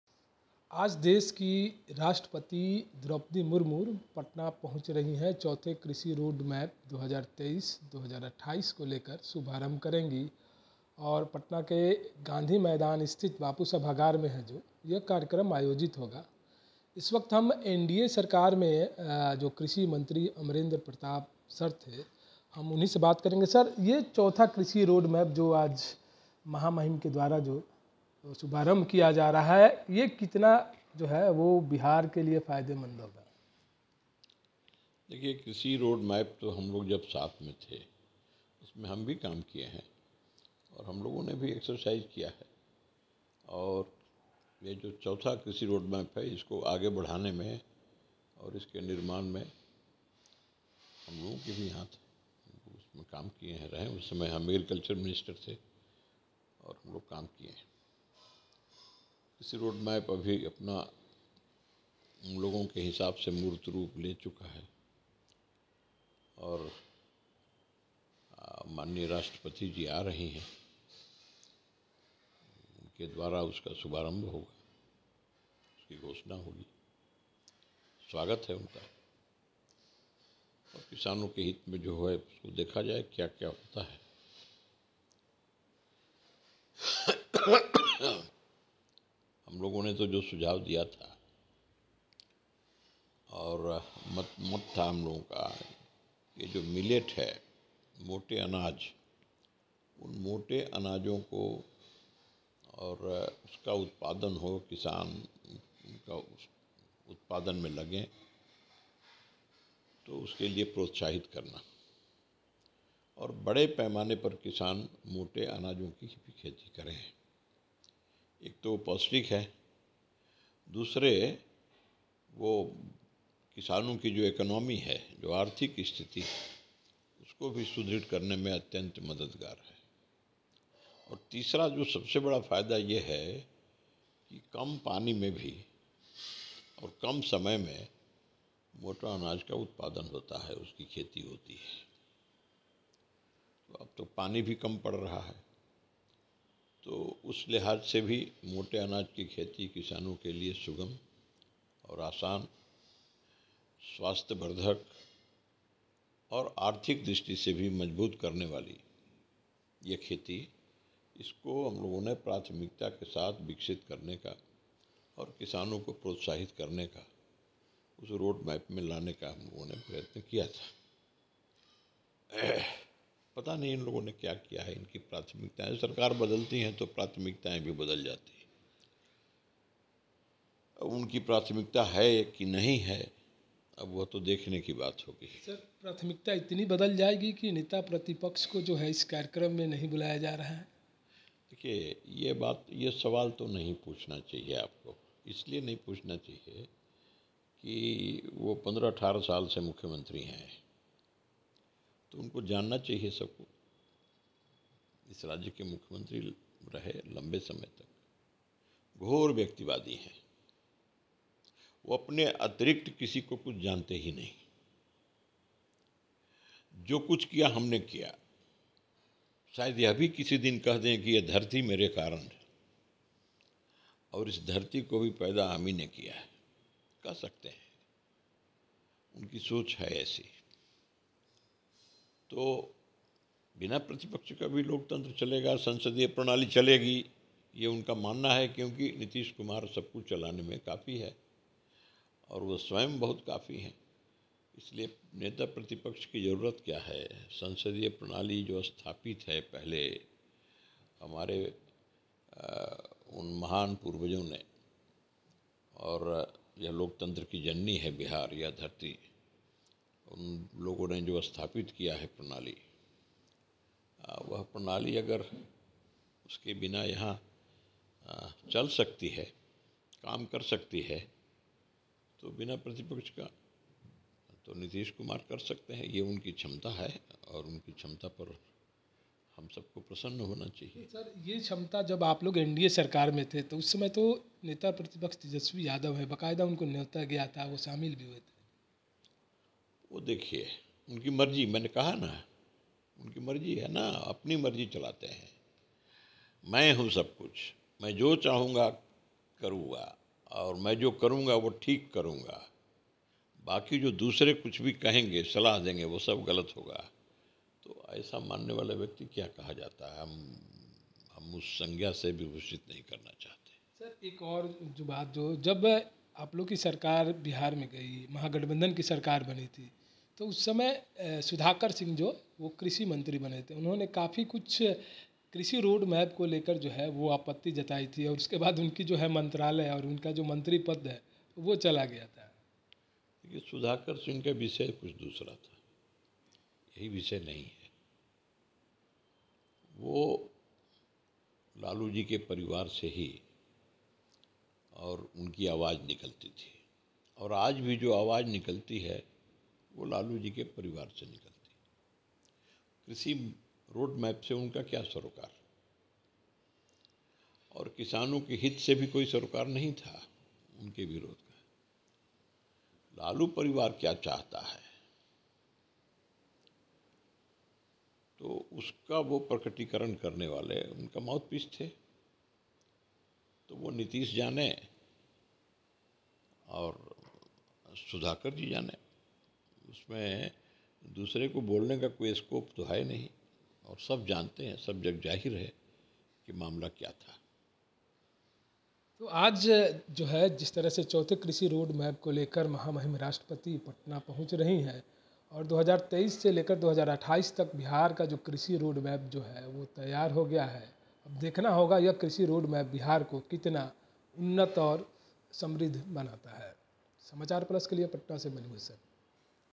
नीतीश कुमार यह भी बोल सकते हैं कि यह धरती उनकी वजह से है - अमरेंद्र प्रताप सिंह
About this Video :- नीतीश कुमार यह भी बोल सकते हैं कि यह धरती उनकी वजह से है - अमरेंद्र प्रताप सिंह